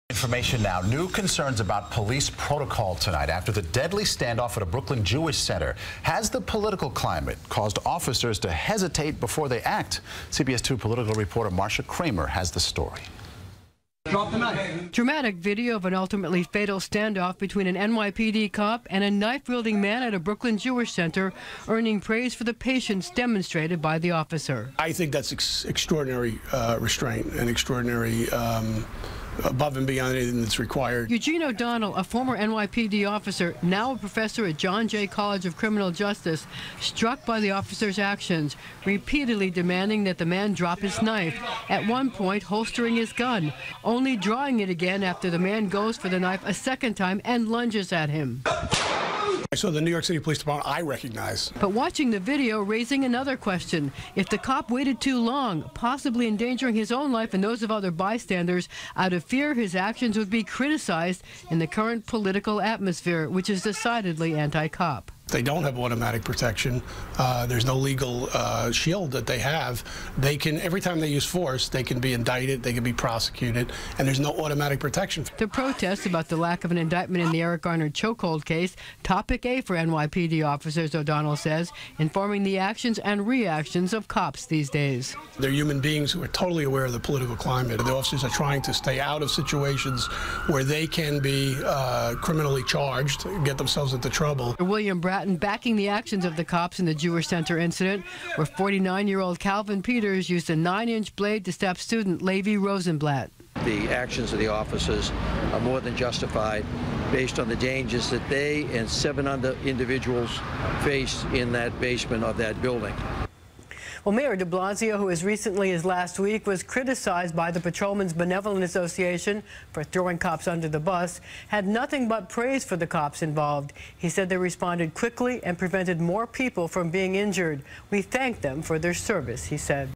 Is Anti Police Climate Making Cops More Hesitant In Dangerous Situations
The deadly standoff at a Brooklyn Jewish center is igniting concerns that the current anti-police political climate in New York can cause cops to hesitate before they act.

As CBS2’s Marcia Kramer reported, dramatic cellphone video of the tense confrontation between an NYPD cop and a knife-wielding man at the Chabad-Lubavitch Hasidic world headquarters in Crown Heights on Tuesday morning is earning praise for the patience demonstrated by Officer Roberto Pagan.

In the video, the cop repeatedly demands that the man, Calvin Peters, drop his knife. At one point, Pagan holsters his gun, only drawing it again when the man goes for the knife a second time.

Soon after, an officer fatally shot Pagan after he lunged with the knife at a cop, police said.

“I think that’s extraordinary restraint … above and beyond anything that’s required,” said Eugene O’Donnell, a former NYPD officer and professor at John Jay College of Criminal Justice.

“I saw the New York City Police Department I recognize,” he added.

But watching the video raises another question: Did the officer wait too long, possibly endangering his own life and those of bystanders, out of fear that his actions could be criticized under the current political atmosphere, which is decidedly anti-cop?

“They don’t have automatic protection,” O’Donnell said. “There’s no legal shield that they have. Every time they use force, they can be indicted, they can be prosecuted, and there’s no automatic protection.”

The protests about the lack of an indictment in the case of Eric Garner, who died shortly after being placed in an apparent chokehold by a police officer, has been Topic A for NYPD cops, informing the actions and reactions of police, O’Donnell said.

“They’re human beings and are totally aware of the political climate,” O’Donnell said. “The officers are trying to stay out of situations where they can be criminally charged and get themselves into trouble.”

Police Commissioner Bill Bratton backed the actions of the cops in the Jewish center shooting, where Peters, 49, used a 9-inch blade to stab student Levi Rosenblatt. The 22-year-old victim, who was stabbed in the left temple, was taken to the hospital in stable condition, police said.

“The actions of the officers are more than justified based on the dangers that they and seven other individuals faced in that basement of that building,” Bratton said.

Mayor Bill de Blasio, who as recently as last week was criticized by the Patrolmen’s Benevolent Association president for throwing cops “under the bus,” had nothing but praise for the police officers involved.

He said they responded quickly and prevented more people from being injured.

“We thank them for their service,” the mayor said.